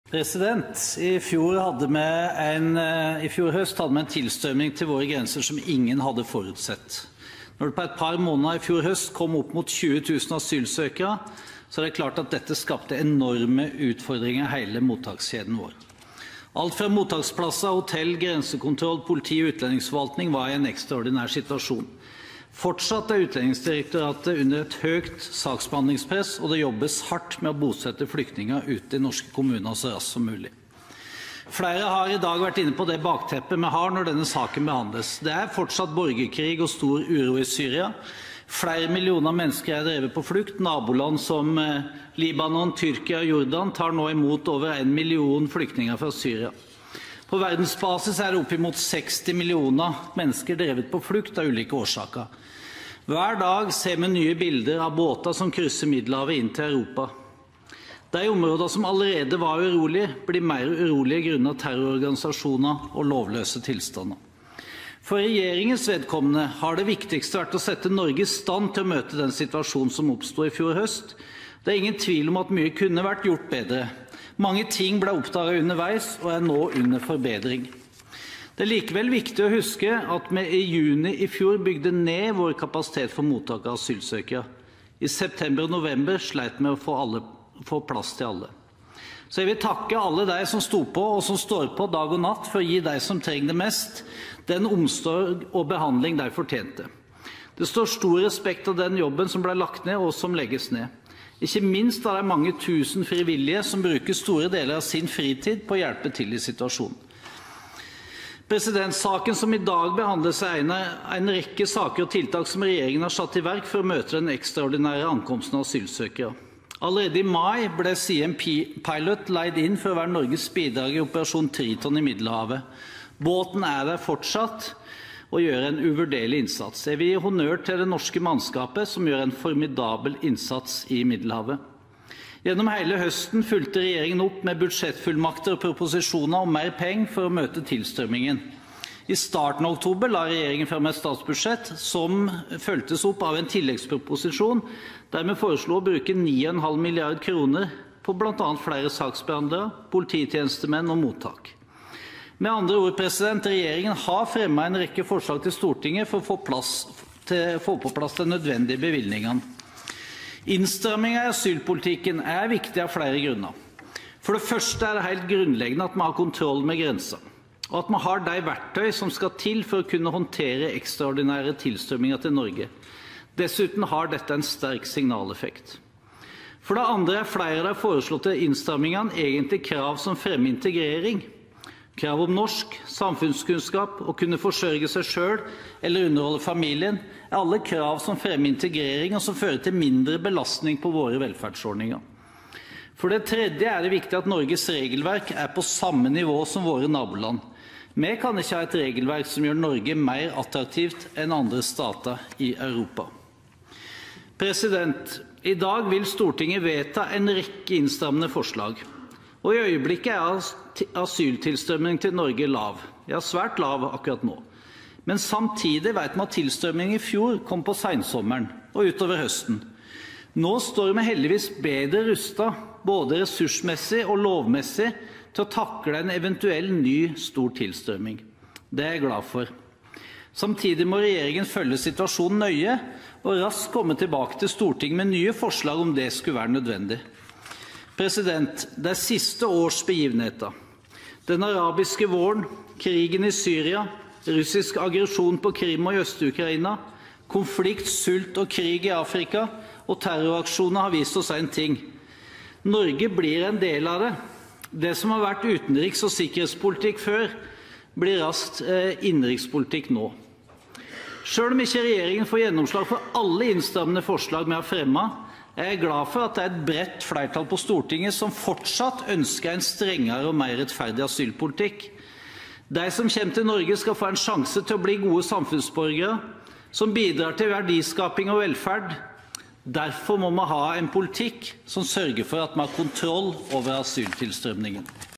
10.06.2016 Sak 1 6 Trond Helleland
Om Endringer i utlendingsloven mv. (innstramninger II) og (pågriping og fengsling i samband med 48-timarprosedyren)